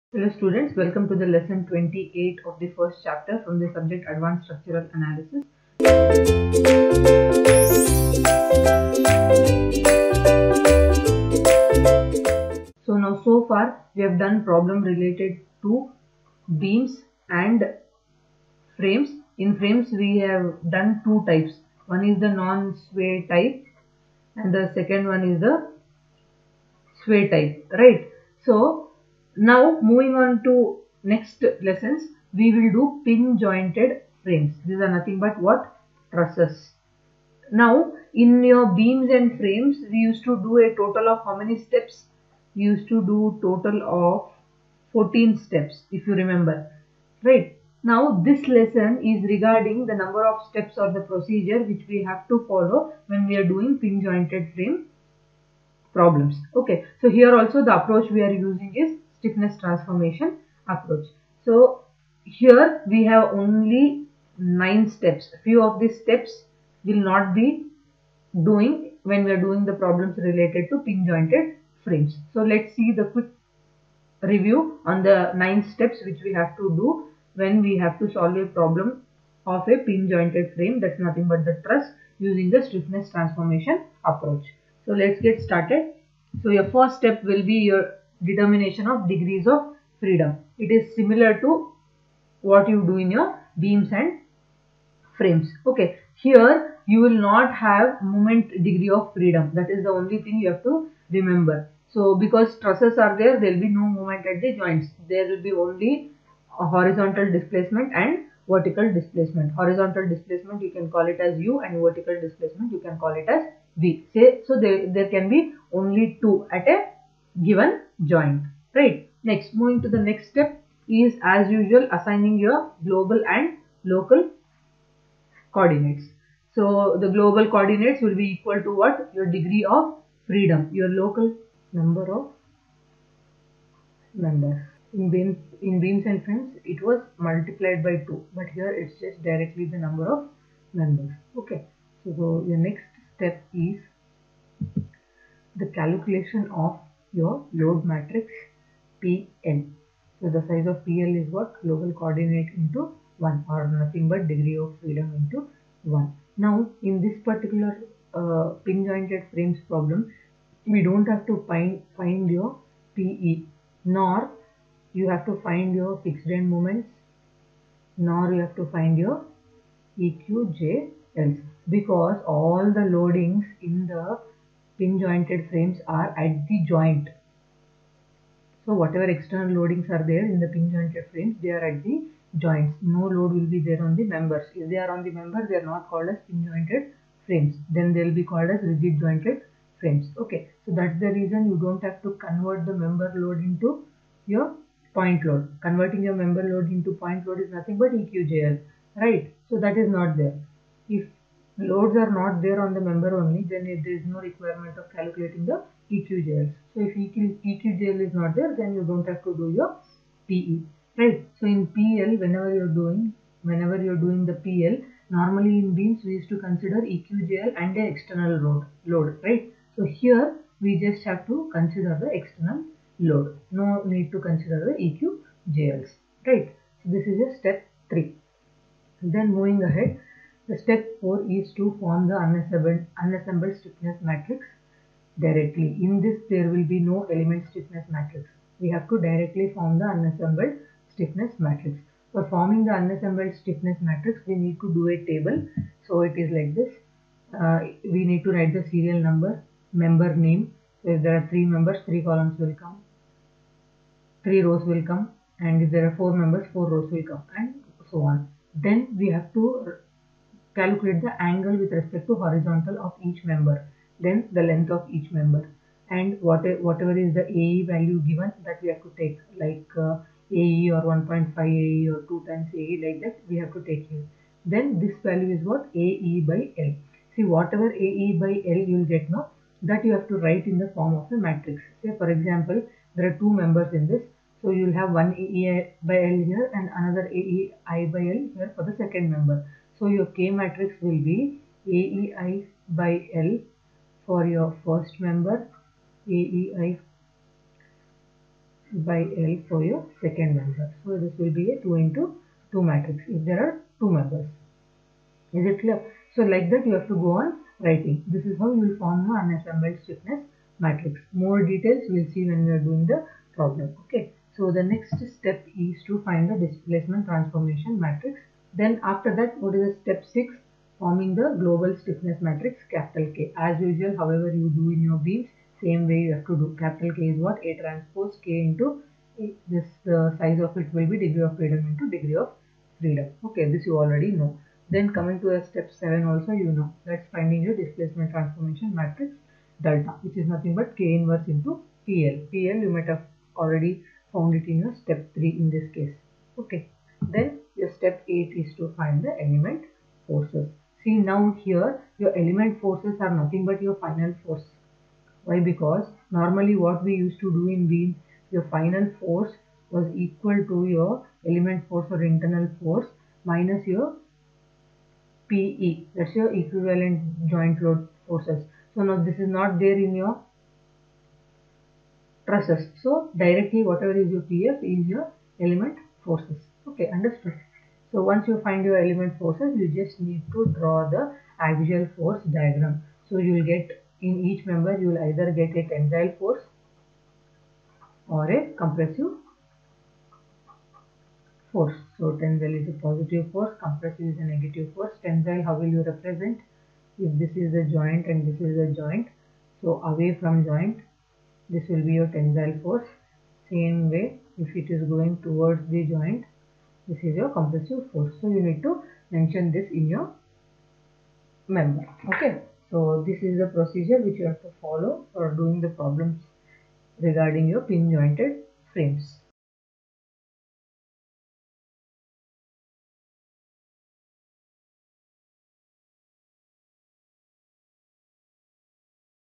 Problem STA Pin Jointed Frames or Trusses Procedure - Advanced Structural Analysis
Subject - Advanced Structural Analysis

Video Name - Problem STA - Pin Jointed Frames or Trusses - Procedure

Chapter - Introduction to Stiffness Method in Matrix Form

Faculty - Prof. Prakruthi Gowd

Access the Complete Playlist of Subject Advanced Structural Analysis -

Happy Learning!